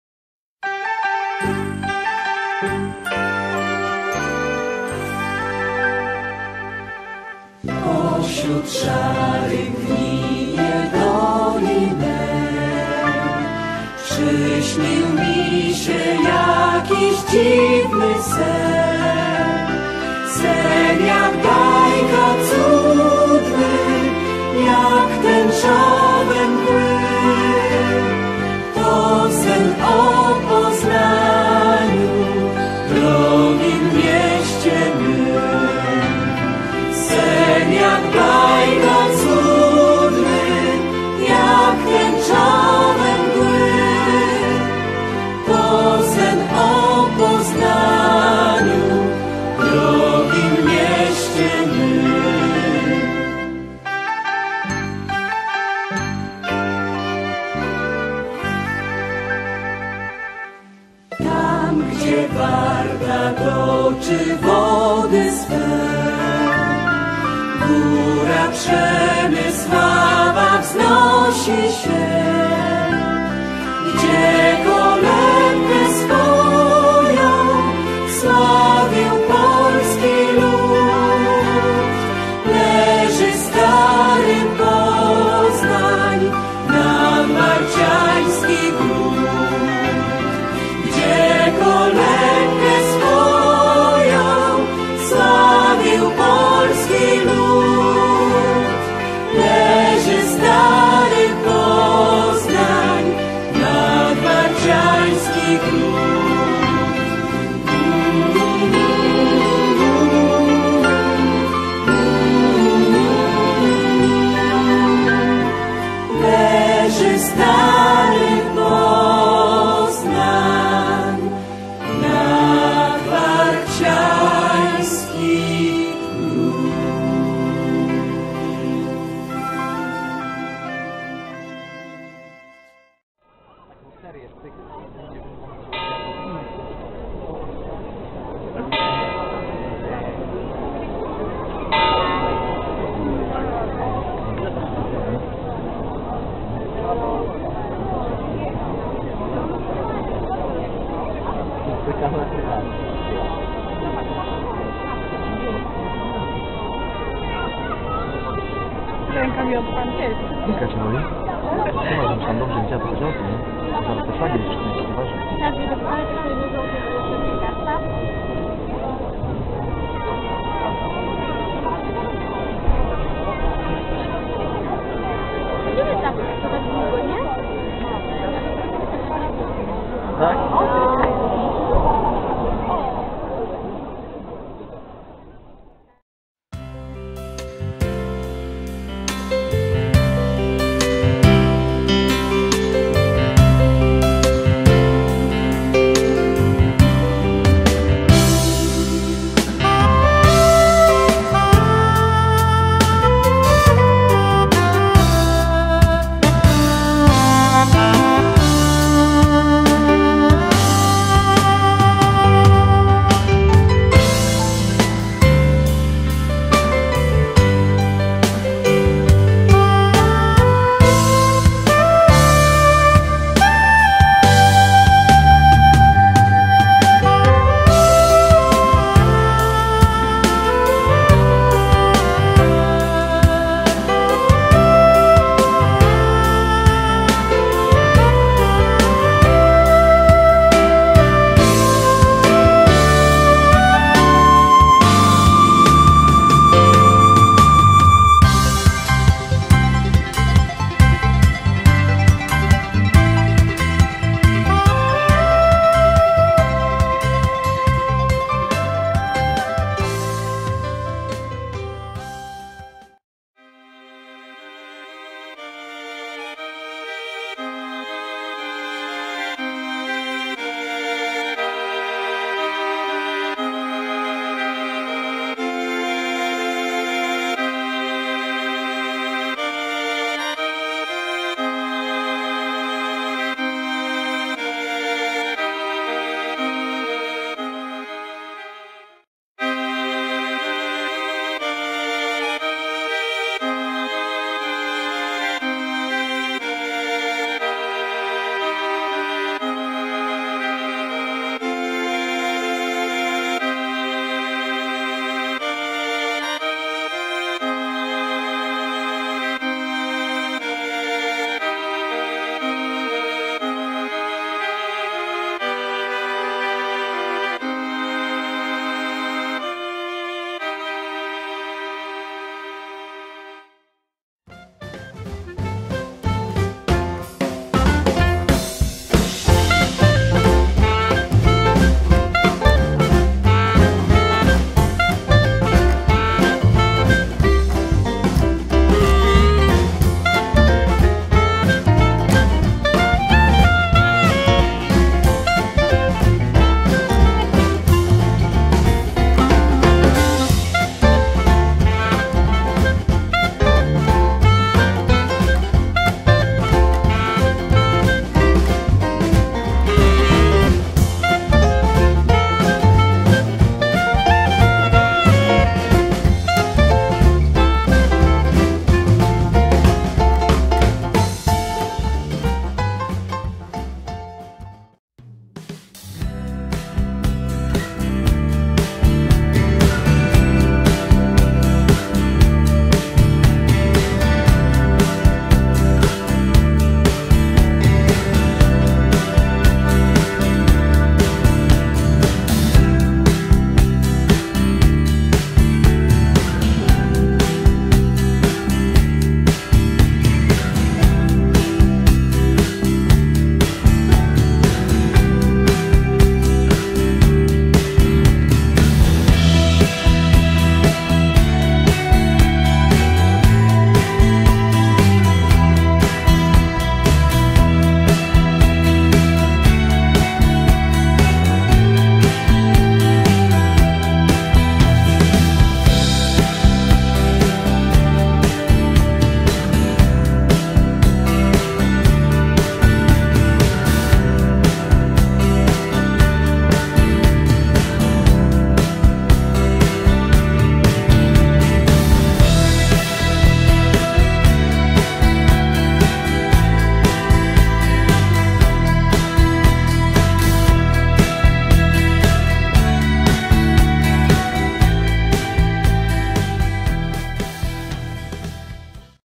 🇵🇱Poznań Polska
Wiosenny spacer po Starym Rynku.